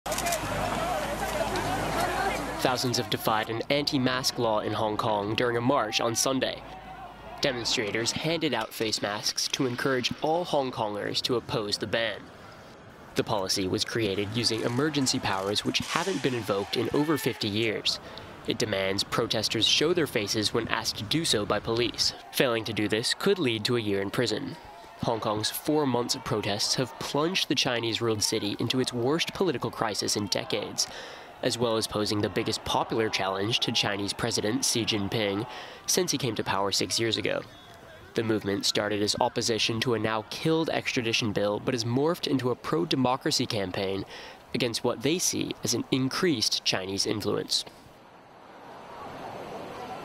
Thousands defy anti-mask law at Hong Kong march
Tens of thousands of protesters marched through central Hong Kong on Sunday wearing face masks in defiance of colonial-era emergency powers which threaten them with a year in prison for hiding their faces.…